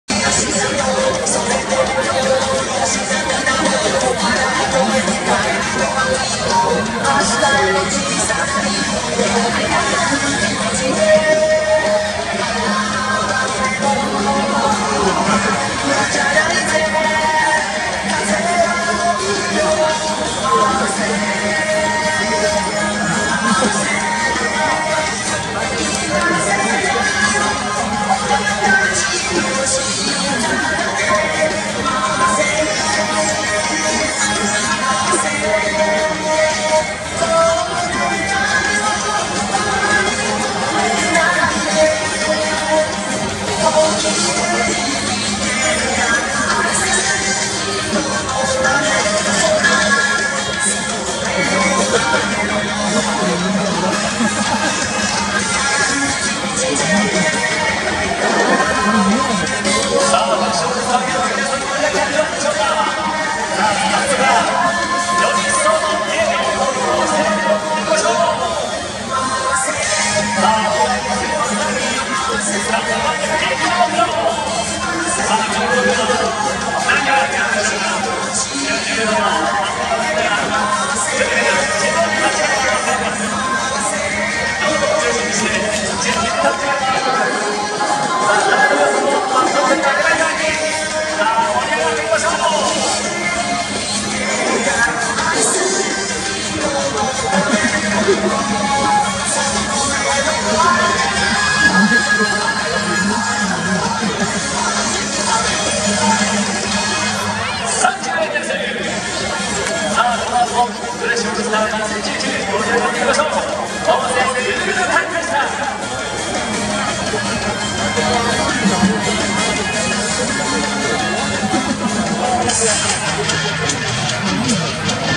つばみのうちわを見て凍り付くマーくんｗ フレッシュオールスター2019 7.11
5回裏終了後のイベントタイム
何かを回すってことでロッテの紙袋を持って来て、コアラのマーチやトッポを出してたマーくんですが、次に取り出したうちわを見て凍り付くｗつばみちゃんが仕込んでたみたいで婚前旅行などと書いてあったようです(*´ω｀)なんだかんだ仲良くしてました！